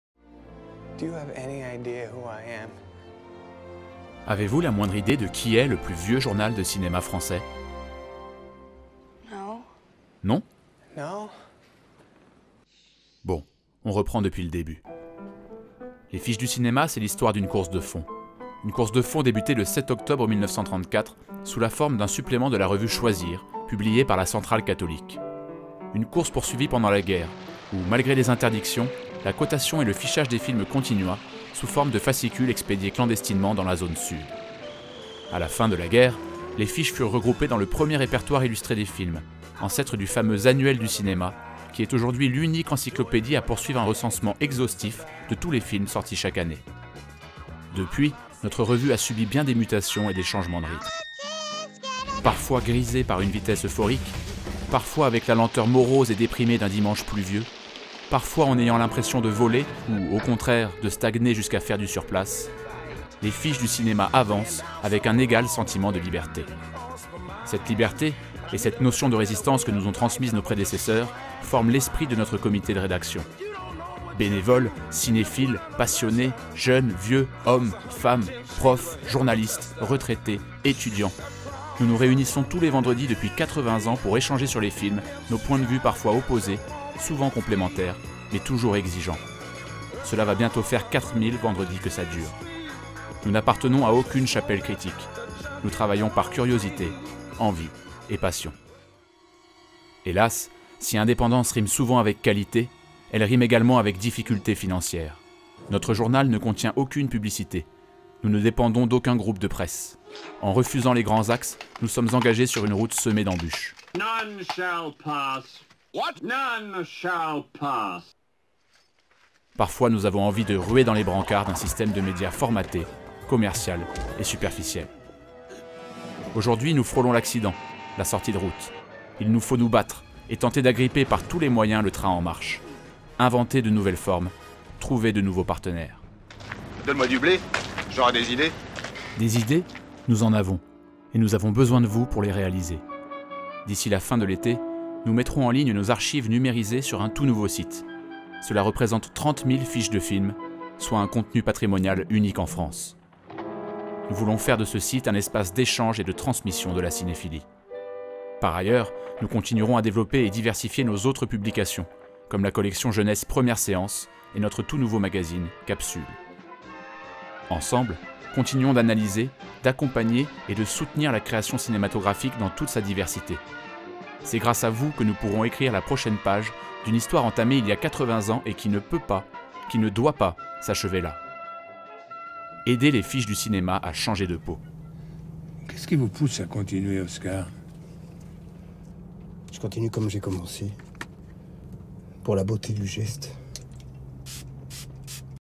Les Fiches du Cinéma font peau neuve
Les éditions des Fiches du Cinéma, fondées en 1934, entament leur révolution numérique en créant une plateforme critique de 30 000 fiches films et en poursuivant, en ligne et sur papier, leur travail d'archiviste du cinéma mondial.

Le 12 avril s'ouvre l'appel à un financement participatif, afin de mener à bien ce projet titanesque et fédérer une communauté cinéphile passionnée, engagée et éclectique.